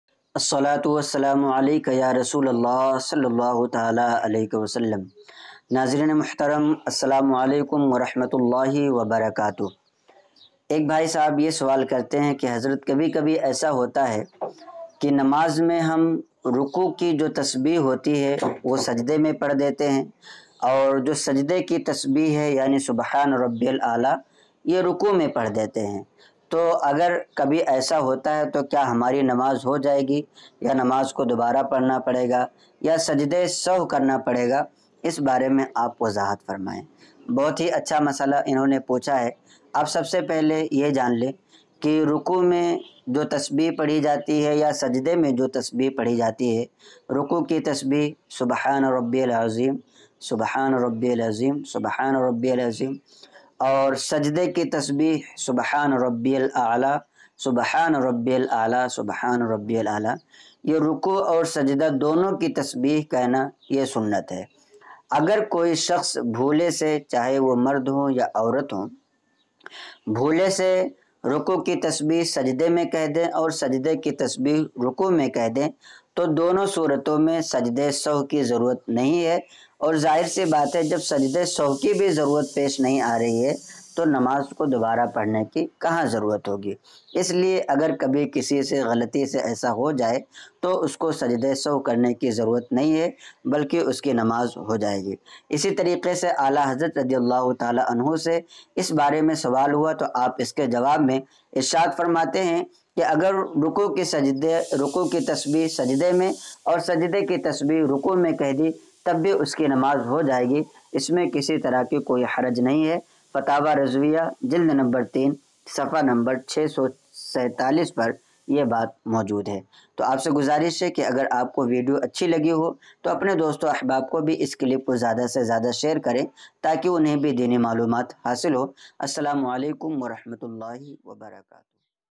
Ruku ki tasbeeh sajde me aur sajde ki tasbeeh ruku me pdh diya to namaz hogi ya nahi
Apne Question WhatsApp Karen